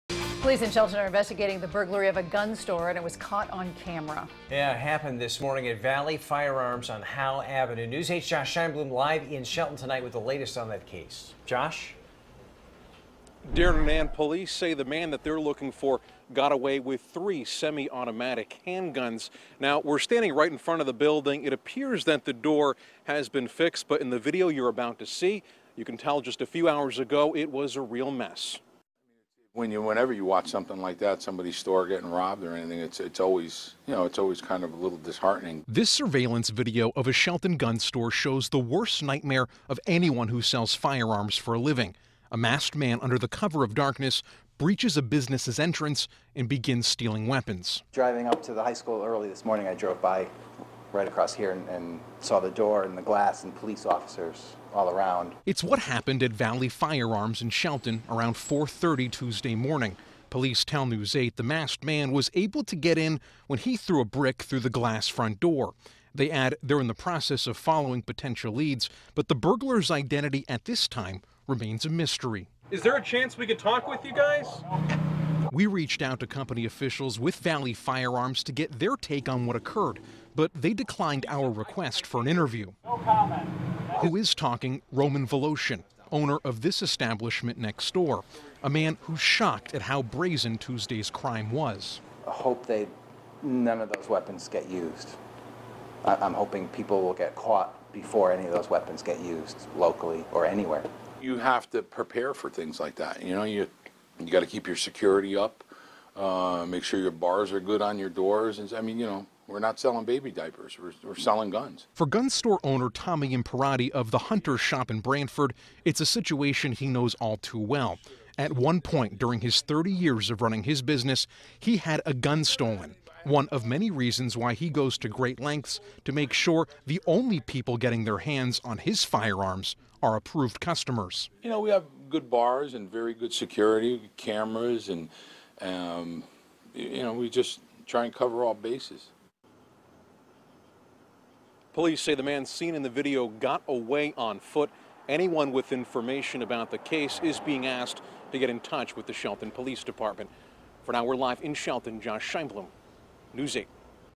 Shelton police release surveillance video of gun store burglary
Shelton police released surveillance video from a local gun store that shows the worst nightmare of anyone who sells firearms for a living. On the tape, a masked man breaches a business' entrance and begins stealing weapons.